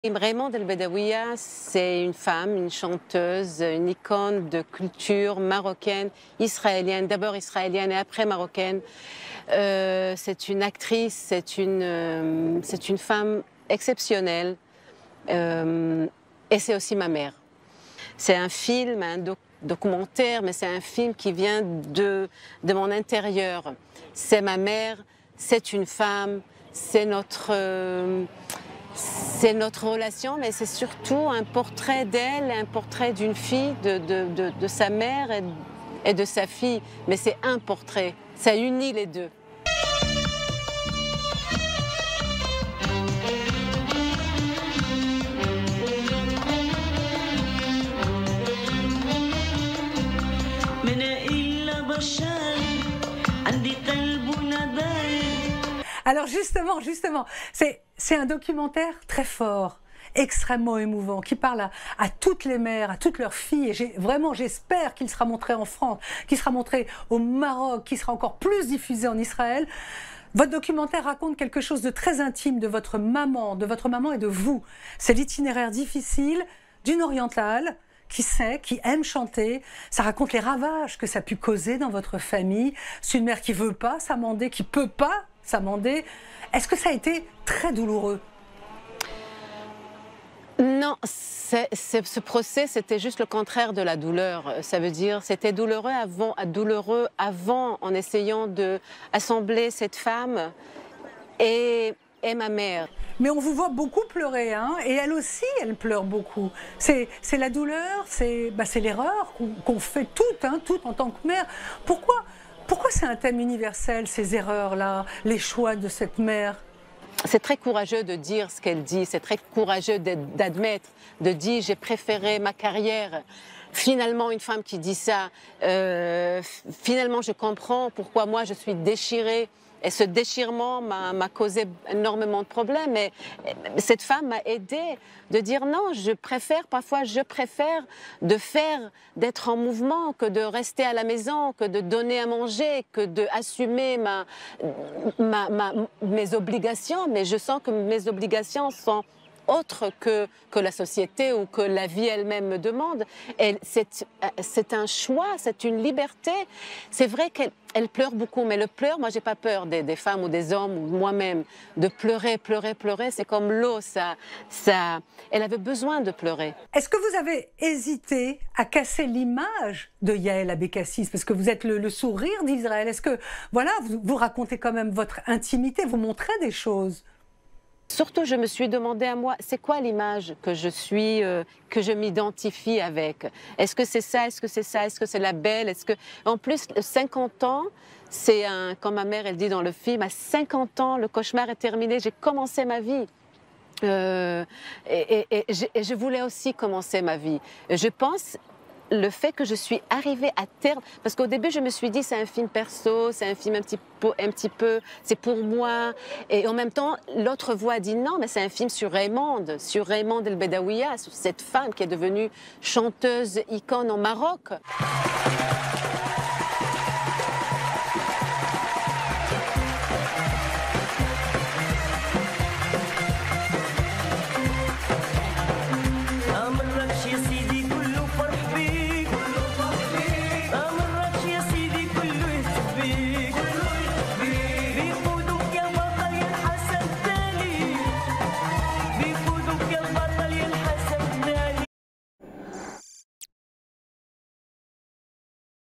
CULTURE | Yaël Abecassis est l'invitée de V. Abécassis
L'actrice, productrice et réalisatrice Yaël Abecassis est l'invitée du magazine Culture au sujet du documentaire "Raymonde" qu'elle a réalisé, et qui porte sur sa propre mère, Raymonde El Abidoia, une chanteuse très célèbre au Maroc qui a sacrifié sa vie de famille pour mener sa carrière.